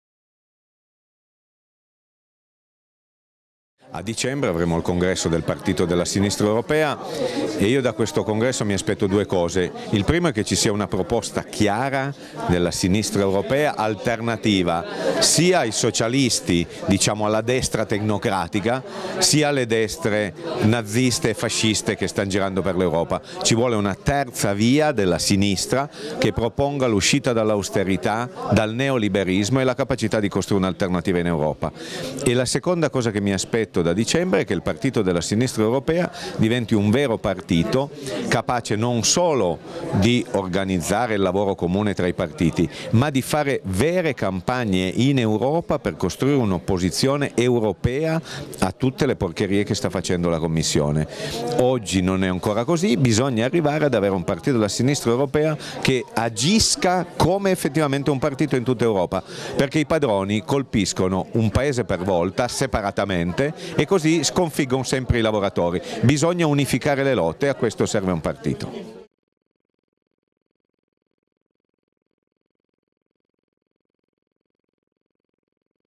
Paolo Ferrero: Towards the Berlin Congress
Paolo Ferrero, leader of the Partito della Rifondazione Comunista (Italy) to shares his hopes and wishes regarding our upcoming Congress in Berlin and the future of Europe.

Subtitles available in English, French and Italian.